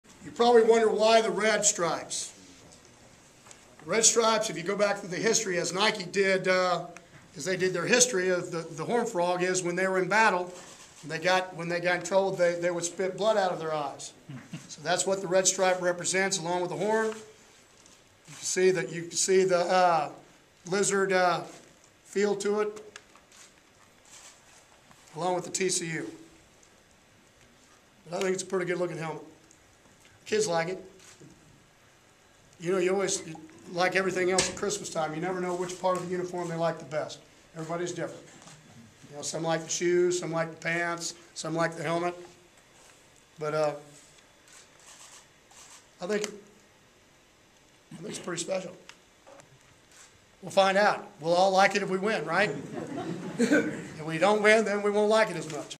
TCU Coach Explains The Red Stripe ... No, Not The Beer
TCU will rock some new uniforms this week against Utah, thanks to some help from Nike. Horned Frogs coach Gary Patterson explained why there are two red stripes on the helmet, and even though it has nothing to do with the popular Jamaican beer, it's pretty interesting nonetheless.